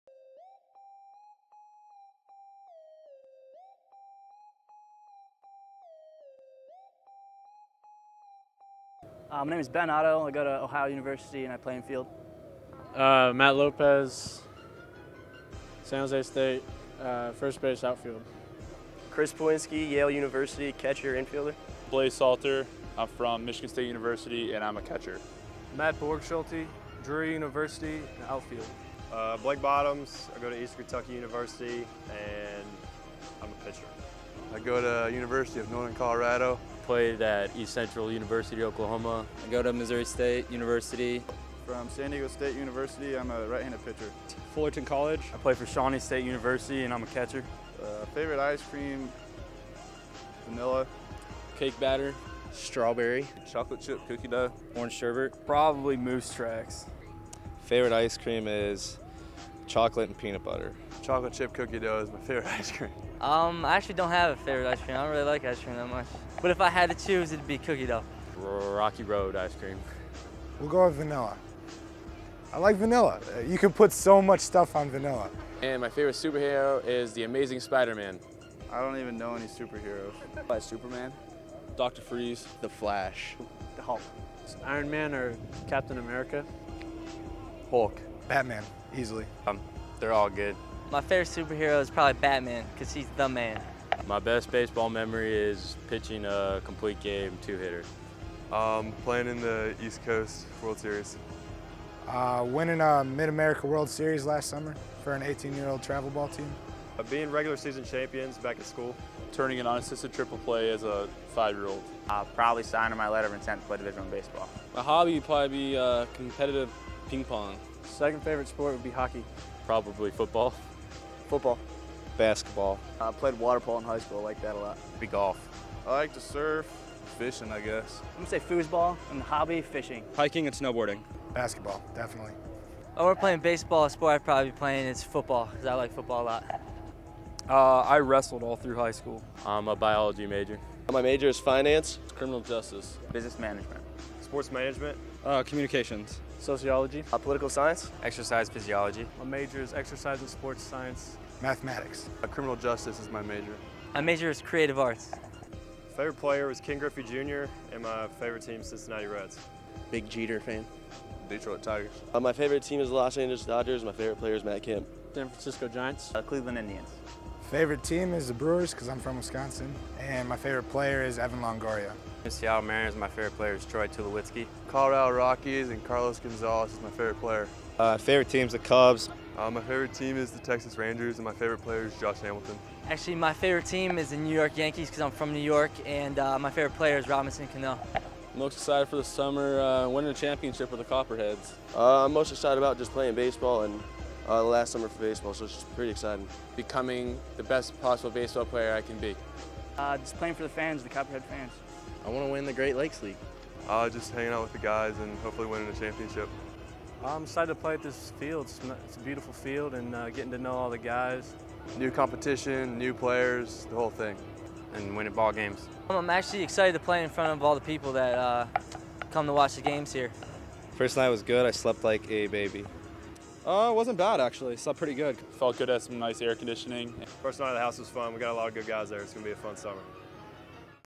Meet The Team 2012
June 12, 2012 - Media Day
Outfield Confessions - Quick blurbs of interesting information to help you get to know out 2012 roster a little bit better.